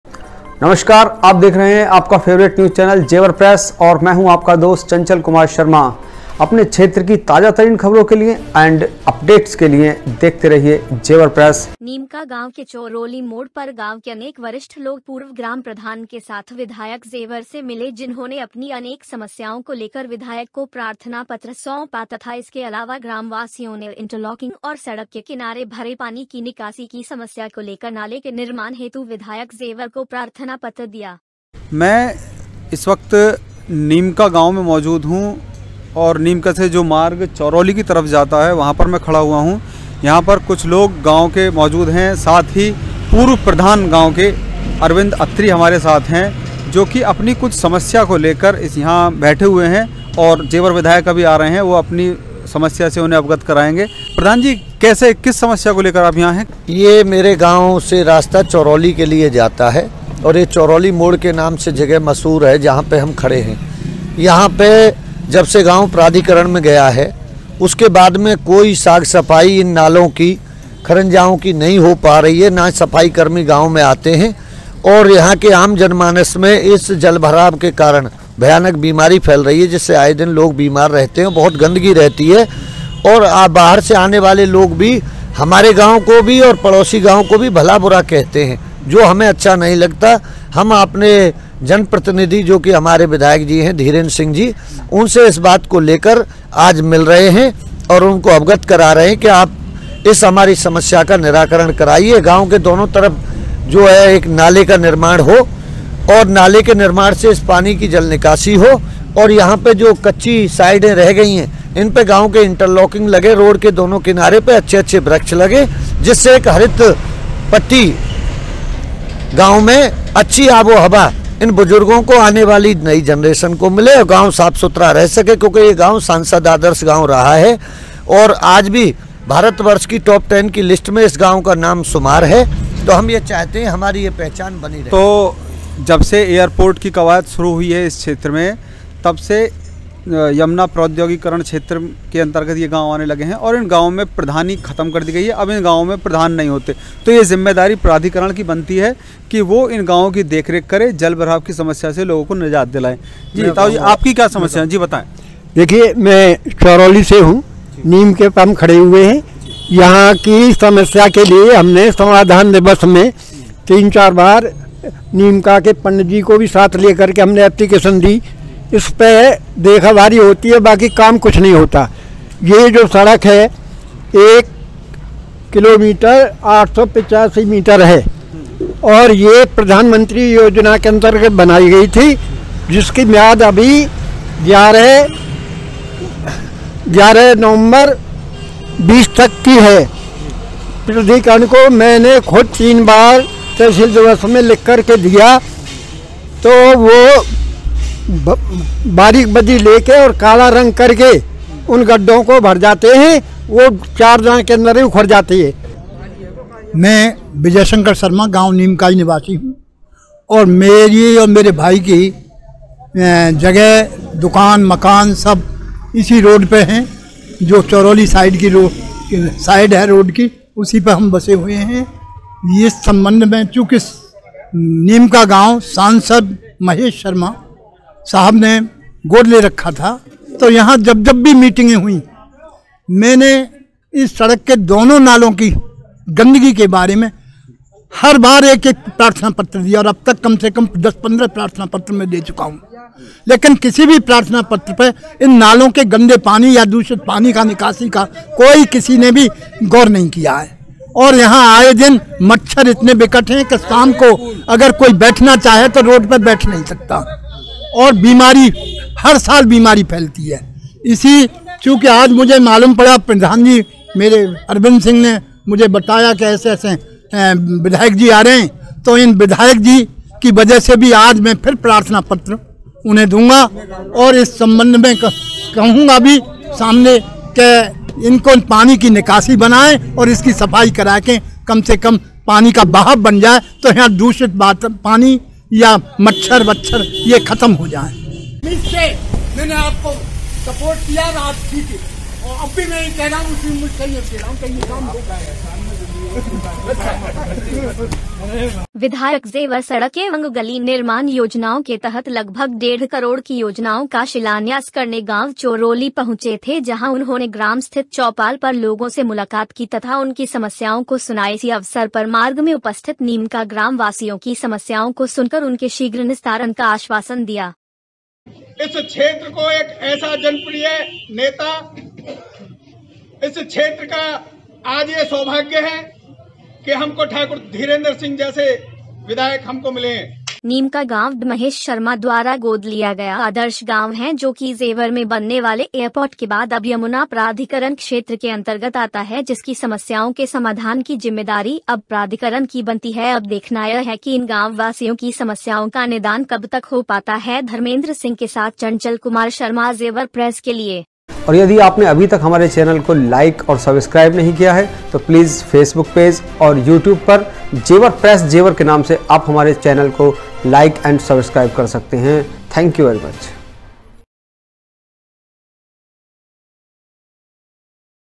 Jewar Press-नीमका-चोरोली _ गाँव के लोगो ने विधायक को बताईं समस्याएँ,
नीमका-चोरोली गाँव के लोगो ने विधायक जेवर को बताईं अपनी समस्याएँ, जेवर विधायक ने शीघ्र निस्तारण करने का दिया अस्वासन,